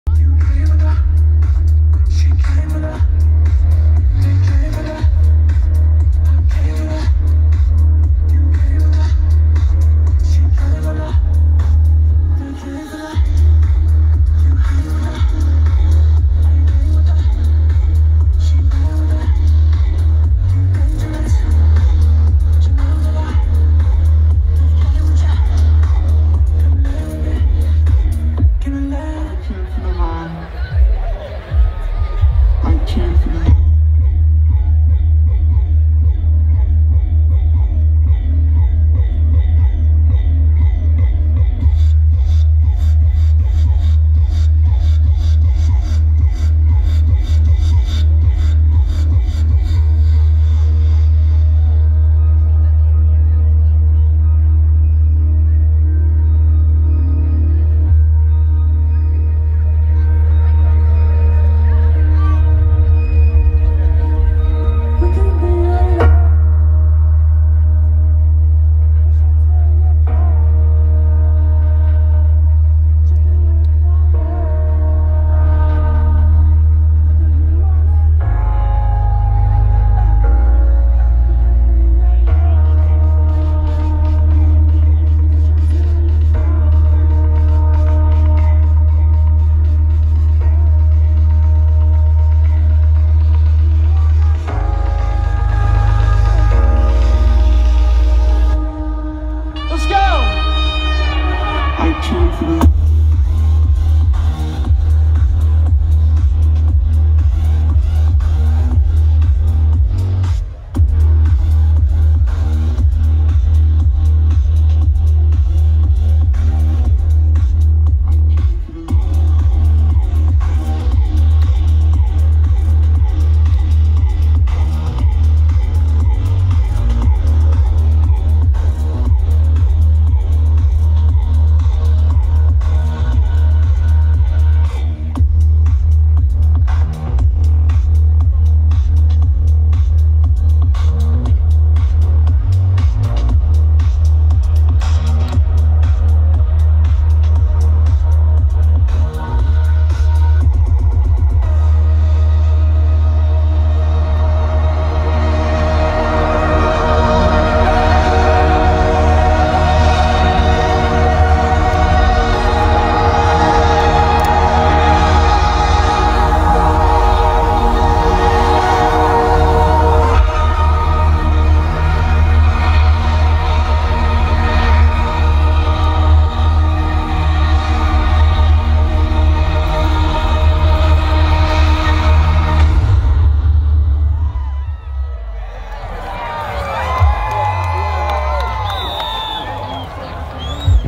Zhu Live at Mainstage at Paradiso Festival 2019 Day 1 - Shot in 4k!
Zhu dropping some amazing live music to get low to ;) Day 1 at Paradiso Festival 2019 Mainstage, shot in 4K with my Samsung S10+.